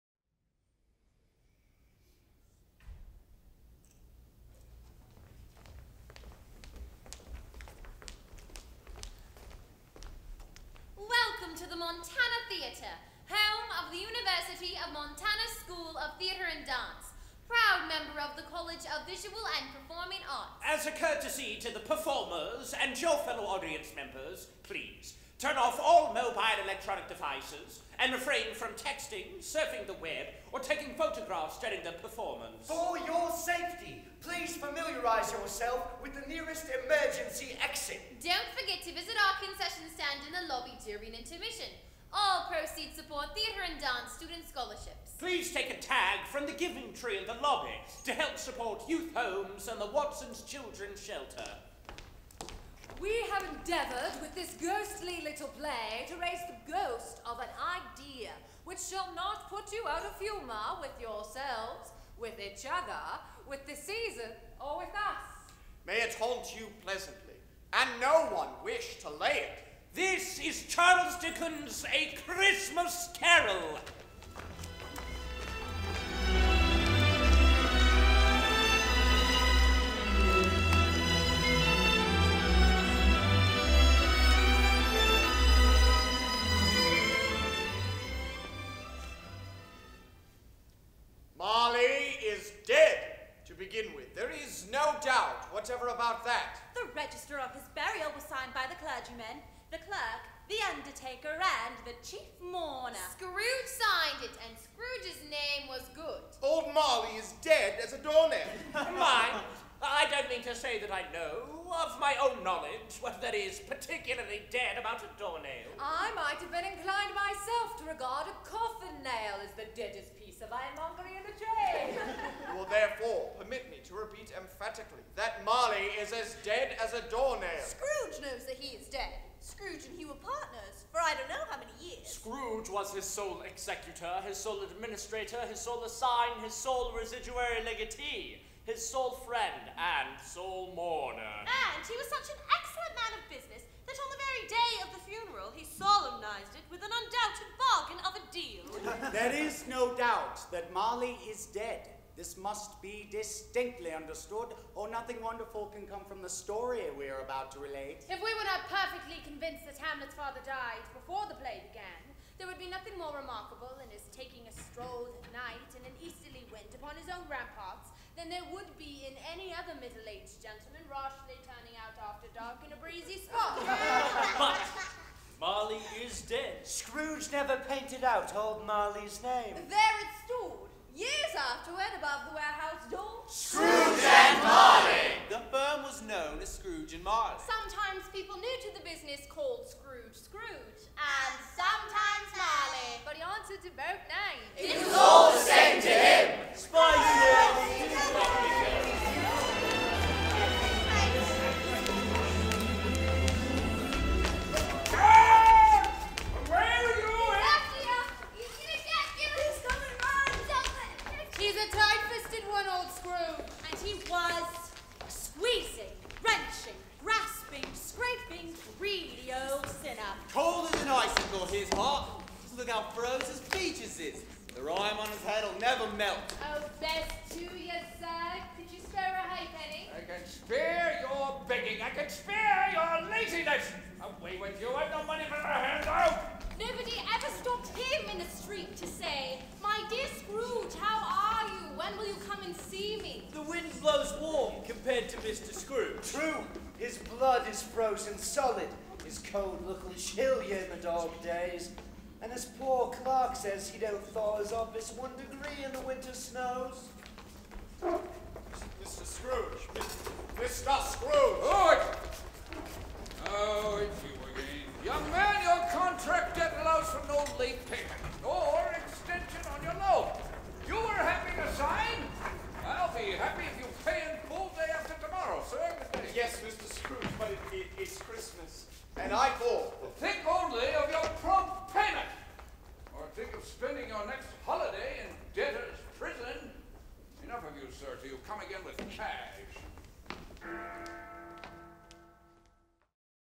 Charles Dickens' A Christmas Carol (Opening)
Opening scenes of University of Montana's production of Charles Dickens' A Christmas Carol directed by Cohen Ambrose